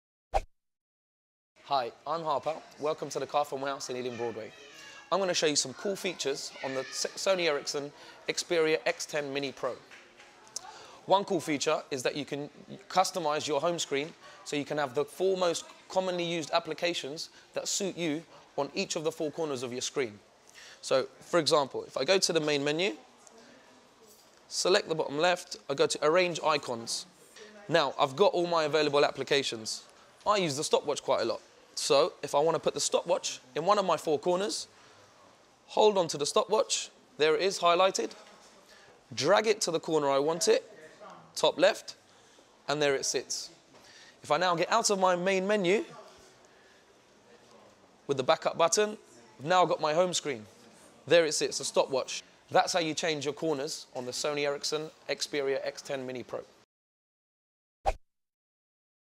Sony Ericsson Xperia X10 Mini - App Hot Corners - The Carphone Warehouse - eye openers
The Xperia X10 Mini allows you to place your 4 most used/favourite apps in the corners of your home screen. To use this feature all you have to do is drag and drop your chosen app into the desired corner on your menu screen.